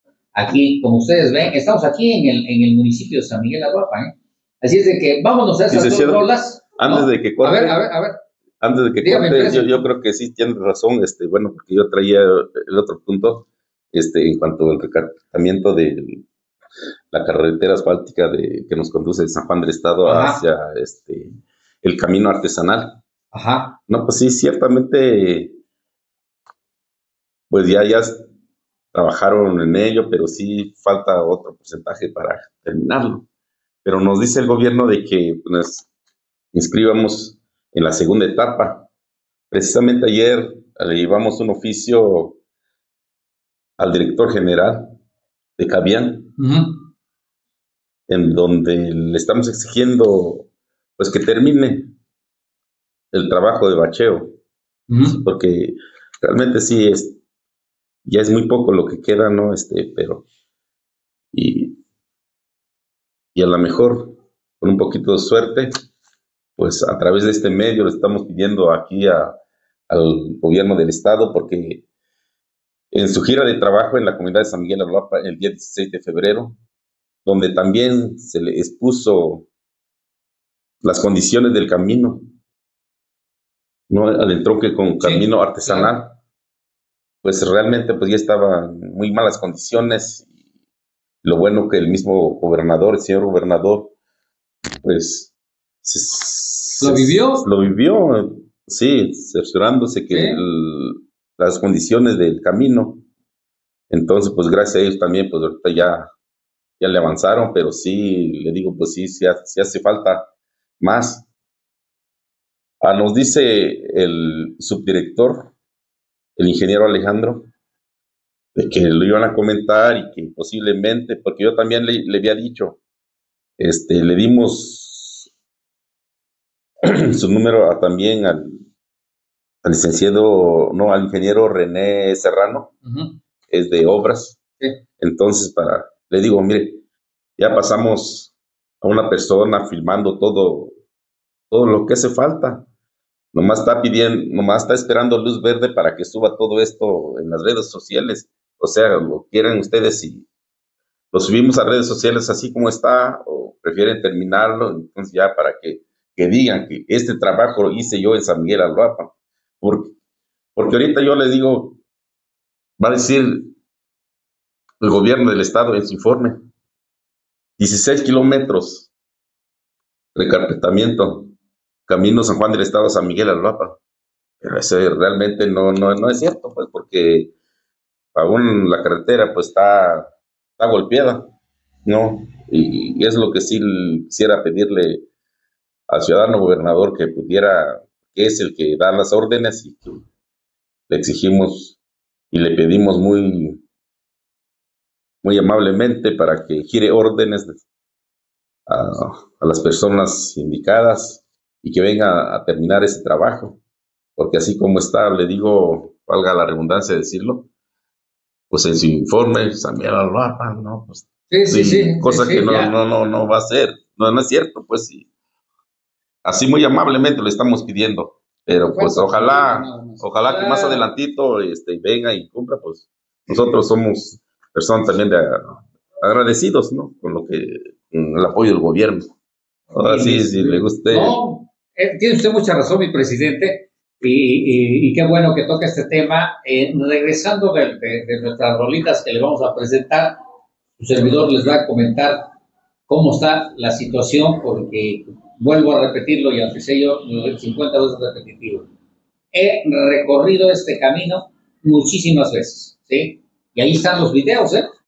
CABIAN no termina trabajos de repavimentacion ya presupuestados para San Miguel Aloapam.
El Presidente Municipal de San Miguel Aloapam, manifiesta que CABIAN y fundamentalmente la constructora, NO CONCLUYO los trabajos de repavimentación y bacheo de la carretera que conduce de San Juan del Estado al entronque con el camino artesanal de San Miguel Aloapam, y el titular de CABIAN argumenta que lo concluiran en una segunda etapa que será apenas presupuestada, sin embargo se tiene conocimiento que todo el proyecto ya estaba firmemente presupuestado. ¿estará enterado de esto el Gobernador del Estado?